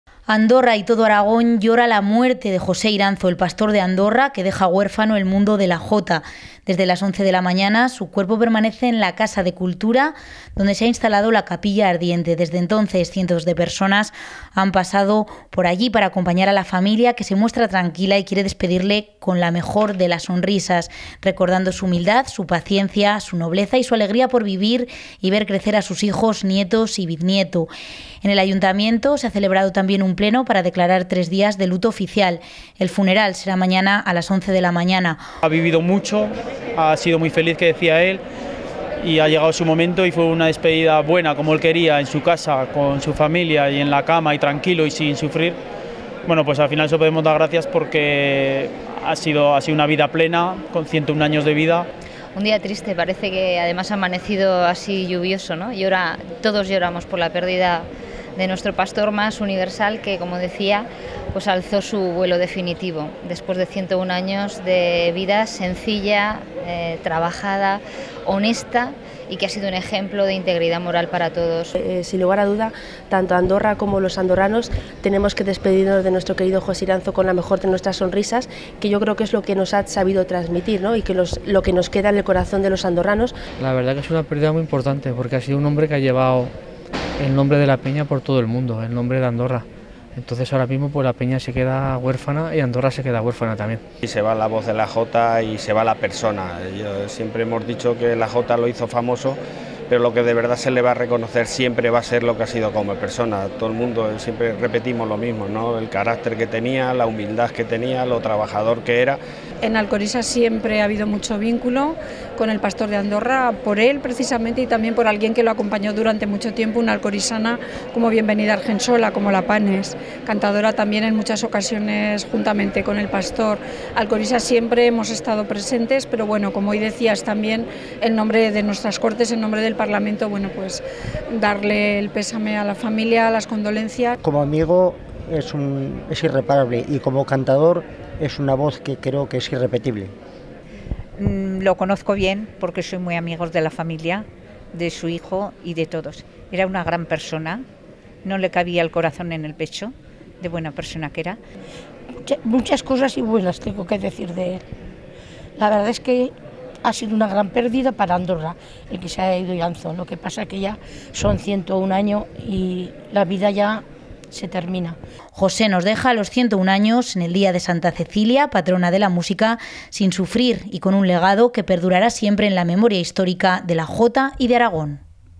La Comarca.tv - Capilla ardiente por José Iranzo
José Iranzo, el Pastor de Andorra, ha fallecido el 22 de noviembre, Santa Cecilia, en su domicilio de Andorra. La capilla ardiente se ha situado en la Casa de Cultura de la Villa minera y cientos de personas han pasado a darle el último adiós. El ayuntamiento ha celebrado pleno extraordinario y ha decretado tres días de luto y las banderas ondean a media asta.